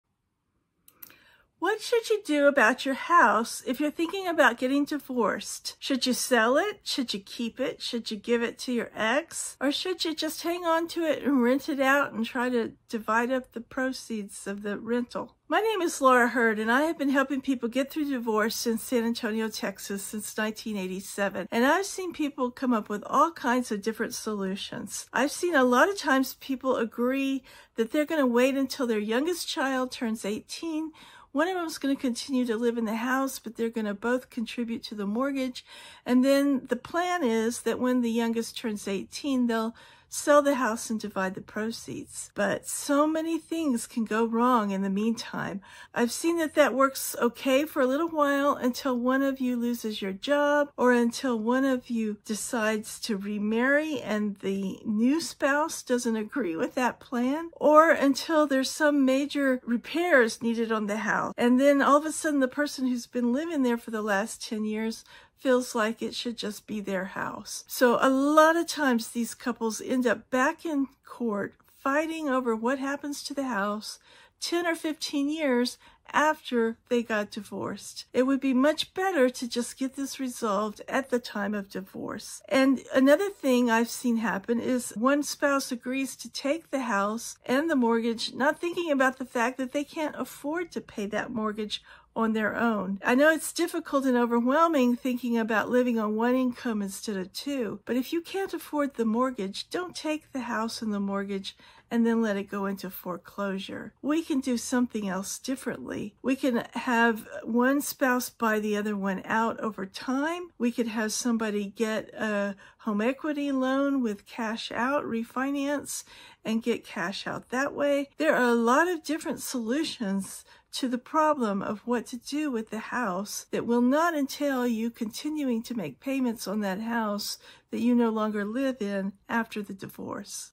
What Happens to the House in a Divorce? Critical Decisions You Need to Make.
Your home is one of the biggest assets in a divorce, but what should you do with it? Sell, keep, or divide? In this video, I discuss the options and potential pitfalls you need to consider when making decisions about your house during a divorce. Make the right choice for your future!

00:00 Introduction: Deciding the Fate of Your House in a Divorce
00:15 Common Solutions and Their Pitfalls
00:42 Challenges of Delaying the Decision
01:46 Alternative Solutions to Consider